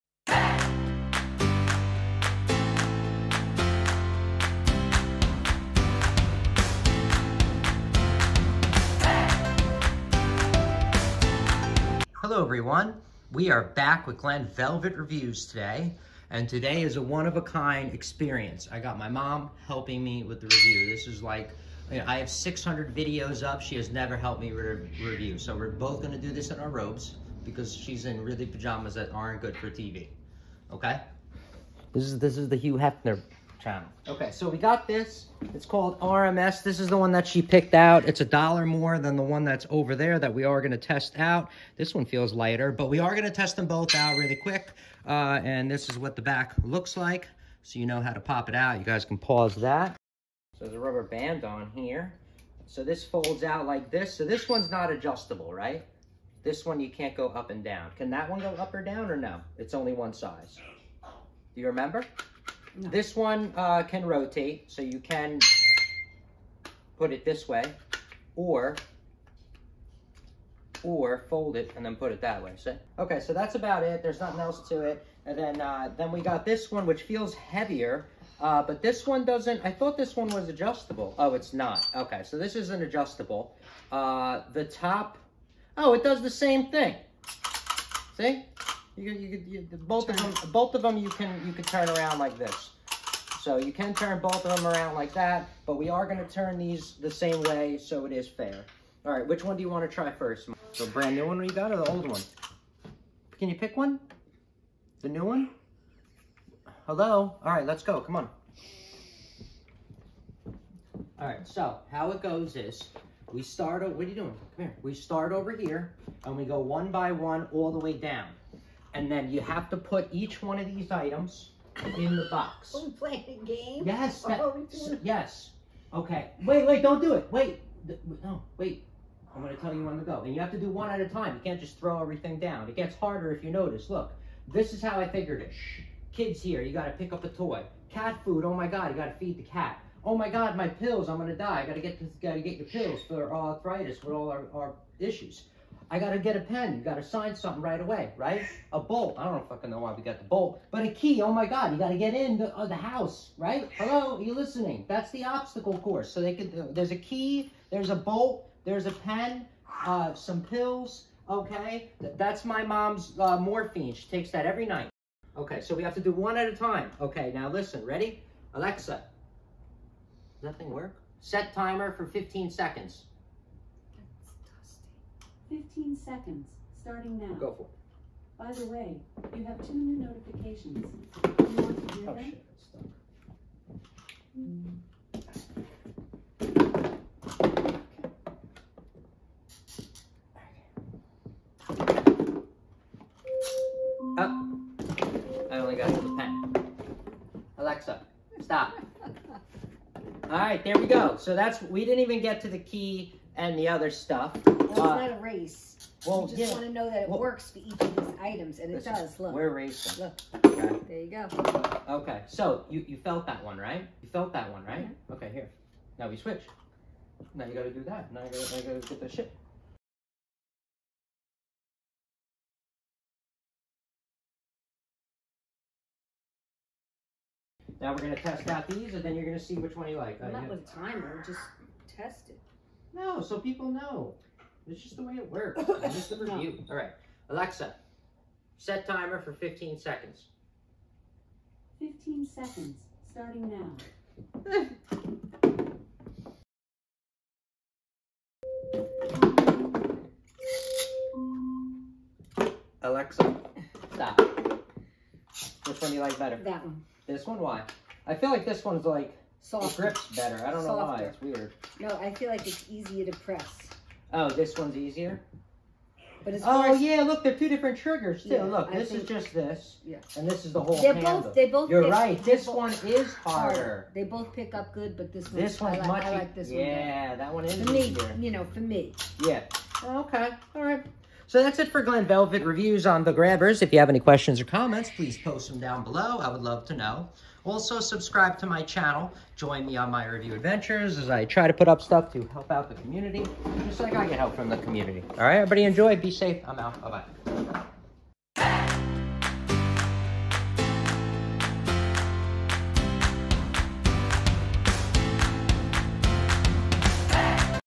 Review of RMS 34 Inch Extra Long Reacher Grabber Vs ZAYAD 31" Reacher Grabber Tool, Mobility Aid
Just want to do a little show down and give our opinionated reviews of these 2 products. I got a new one after my back surgery to help me while I heal. These work great and definitely saved me a few times. Also, if you have the new style extra deep washing machines without a step stool these are perfect.
Any questions or comments please post them down below.

What's Included: One 34" long folding grabber reacher with rotating jaw.
34" LONG: The grabber reacher measures 34 inches long from end to end.

You can pick up items from the floor, top shelf, outdoor lawn, trash can, behind furniture, or other hard to reach places.

Rotating Jaw: The jaw rotates and locks at 90 degrees for vertical or horizontal use. It is made from soft rubber to allow picking up the smallest items with ease.

Durability: The shaft is made from rust-proof lightweight aluminum and the internal wire is made from steel cable for durability and ease of use.

Travel Friendly: The unique folding design makes it easy to fold and store your Reacher when traveling. Now you can take the Reacher with you wherever you go.